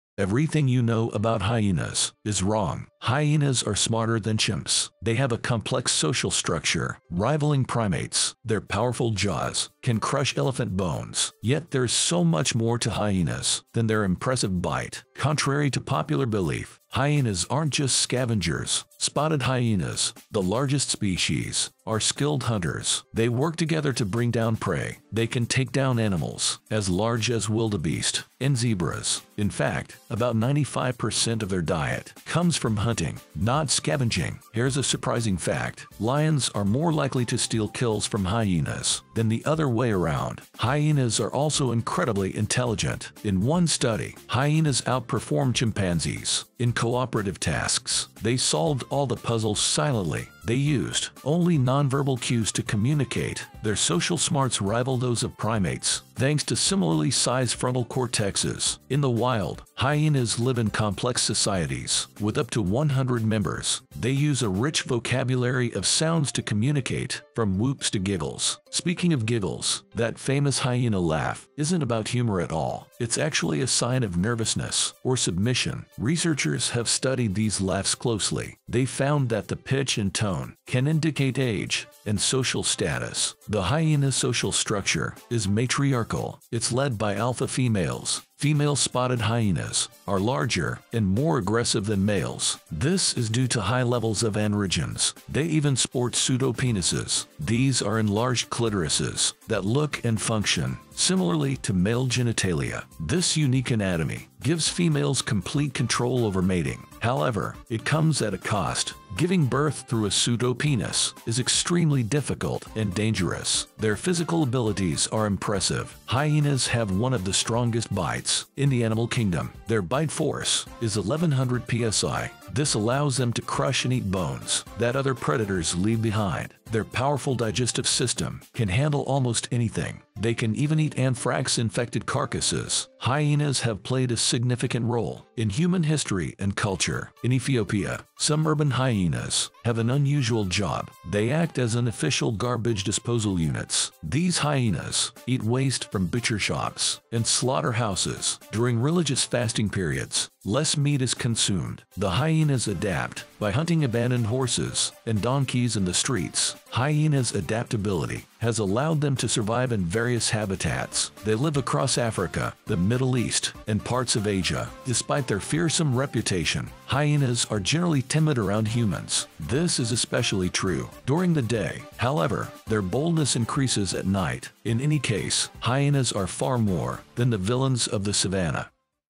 Everything You Know About Hyenas Is Wrong
The myth of hyenas as cowardly scavengers couldn’t be further from the truth.